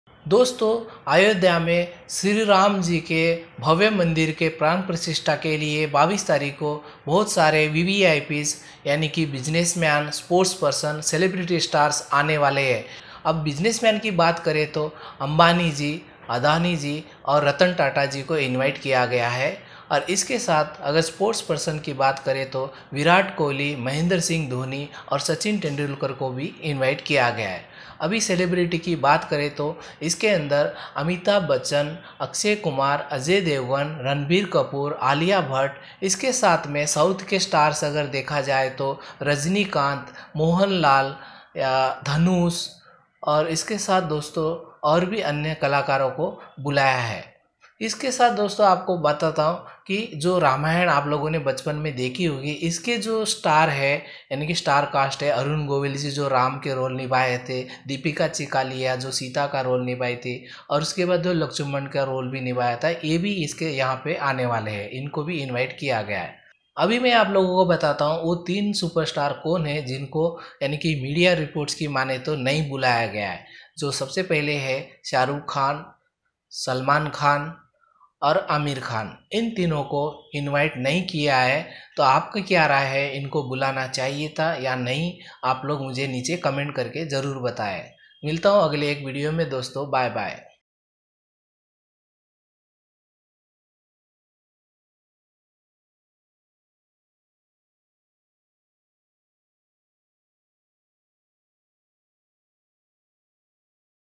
Ayodhya में VVIP लोगों को मिला न्योता
Ayodhya में VVIP लोगों को मिला न्योता | CineFlamer

On the historic day of 22nd January, countless devotees from diverse backgrounds unite for a common purpose—to witness the inauguration of the Ram Mandir in Ayodhya. Joining Prime Minister Narendra Modi in this momentous event will be renowned figures from the film industry. From superstar Rajinikanth to megastar Amitabh Bachchan, and the original on-screen Ram Arun Govil, alongside Sita, Dipika Chikhlia, numerous A-listers and prominent personalities are set to grace the consecration ceremony of the Ram Lalla ido

All eyes are set on the grand ceremony in Ayodhya. Several prominent personalities have been invited. While a few have decided to not attend the event.